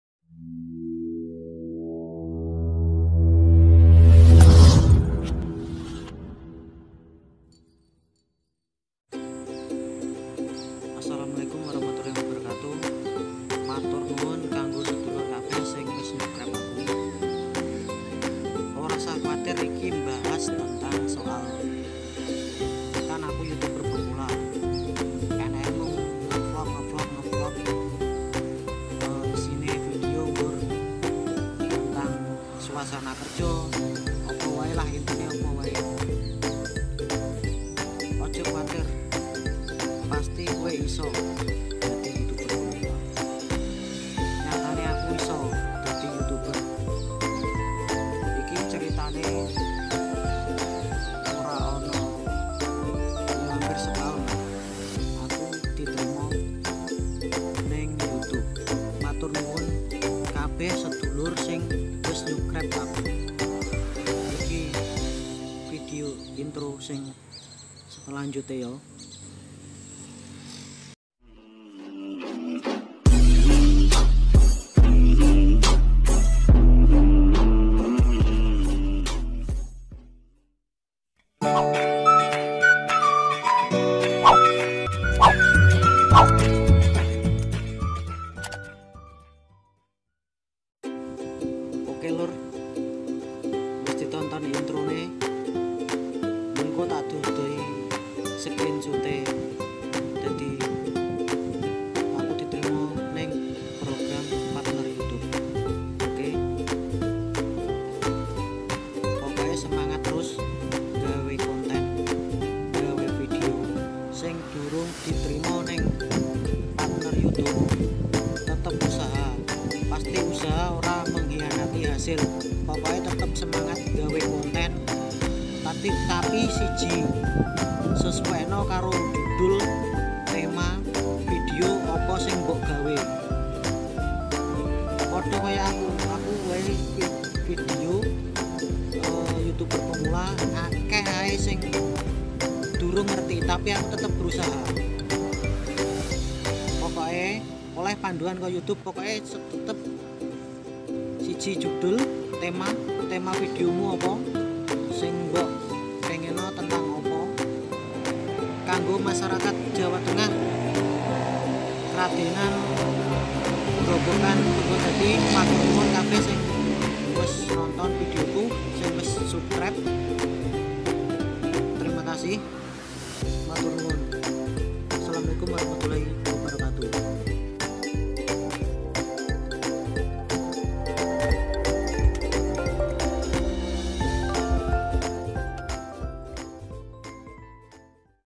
bahas tentang program partnert youtube,vlog ono mancing ono pekerja proyek
bahas tentang program partnert youtube vlog hari ini.
terima kasih buat youtube sudah mau menerima chanel ini.ini hanya chanel hiburan mencakup semua video campuran akan di upload.